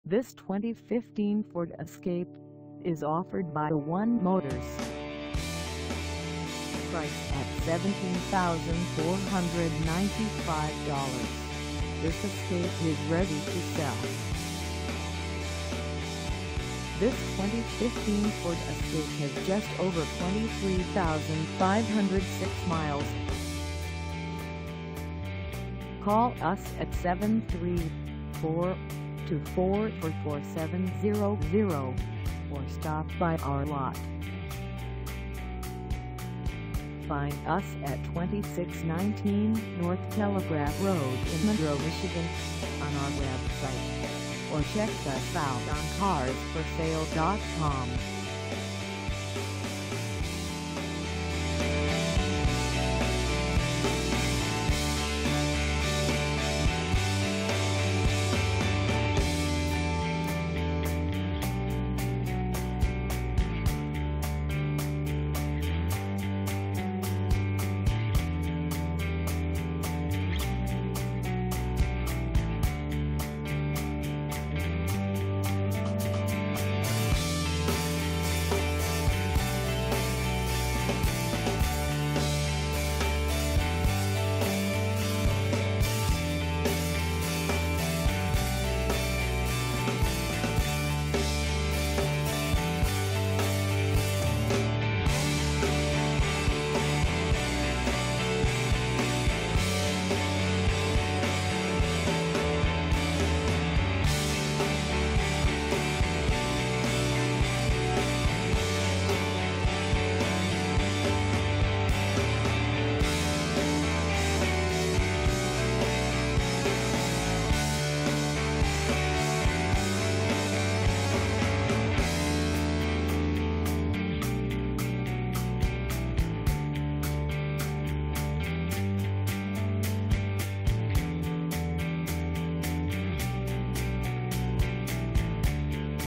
2015 Ford Escape SE AWD 4dr SUV for sale in Monroe, MI 48162
This 2015 Ford Escape SE AWD 4dr SUV is for sale in Monroe, MI 48162 at A 1 Motors.

Engine: 2.0L I4 Turbocharger
Transmission: Automatic 6-Speed
Color: Blue
Mileage: 23506
Contact A 1 Motors 2619 N Telegraph Rd Monroe, MI 48162 to test drive this 2015 Ford Escape SE AWD 4dr SUV today.